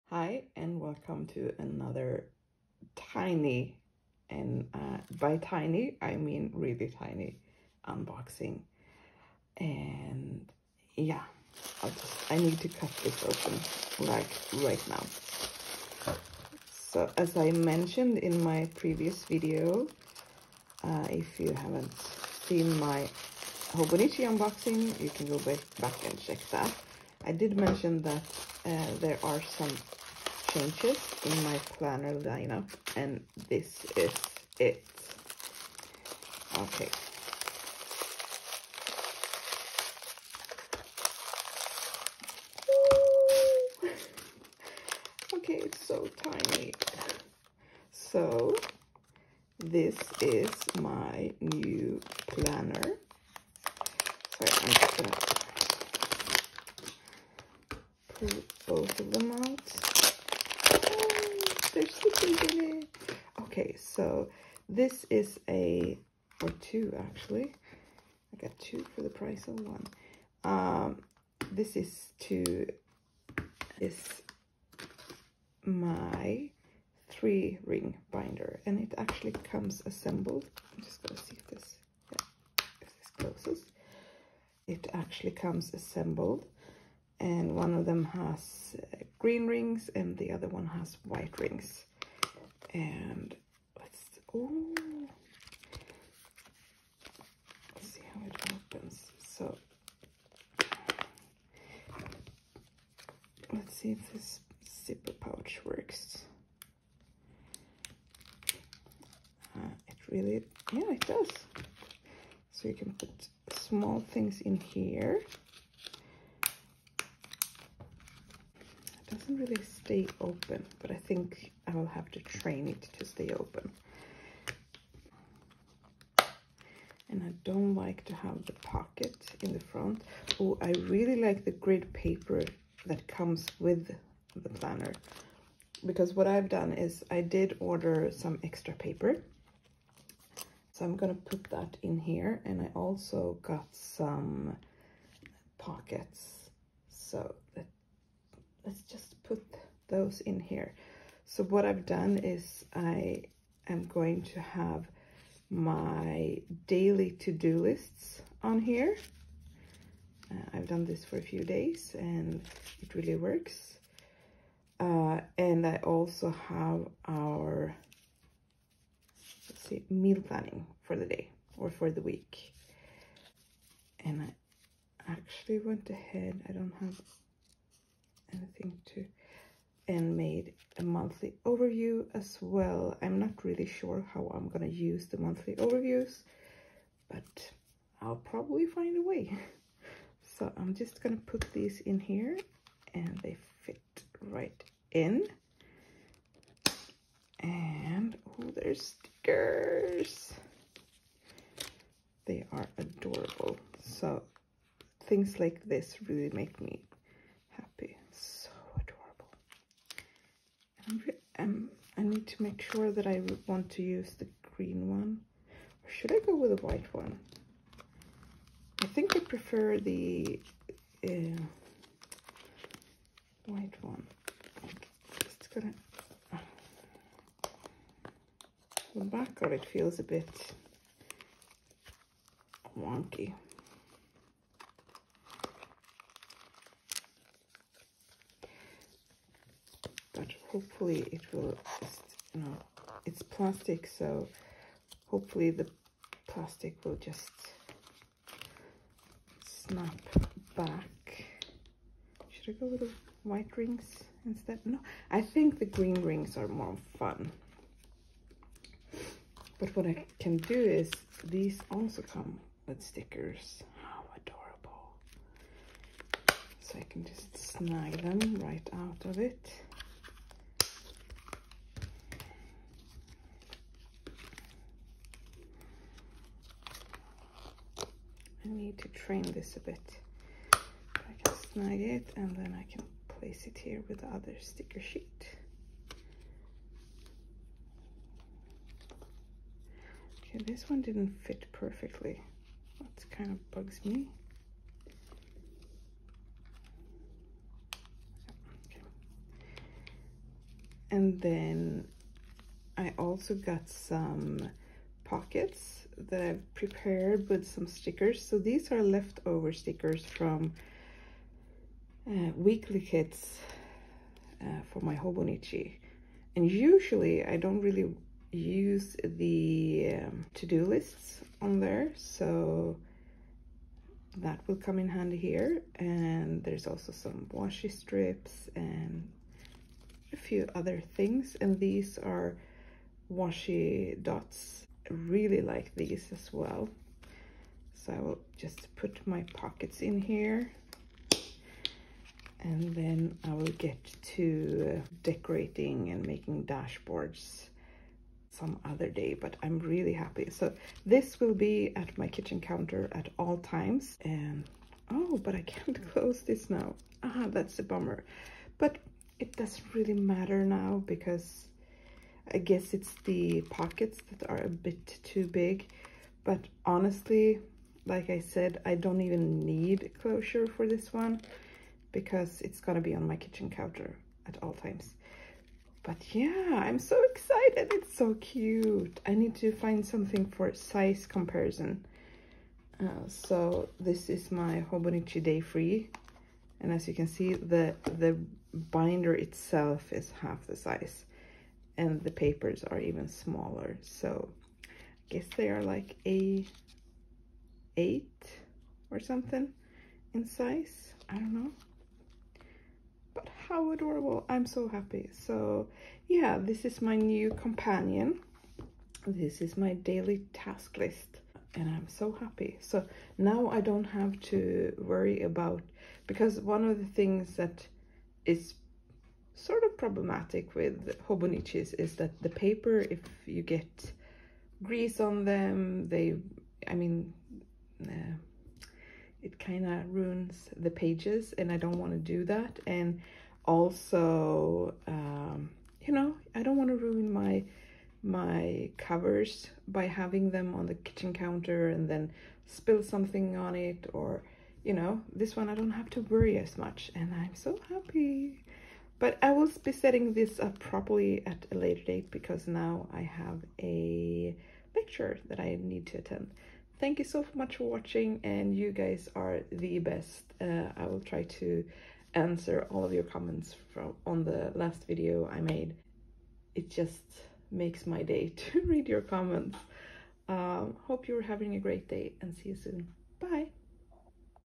3 ring binder unboxing | Teeny tiny planner
Unboxing of a teeny tiny planner. Please let me know if you want to see more in depth how it’s setup or perhaps a setup video.

Take care!
/Anna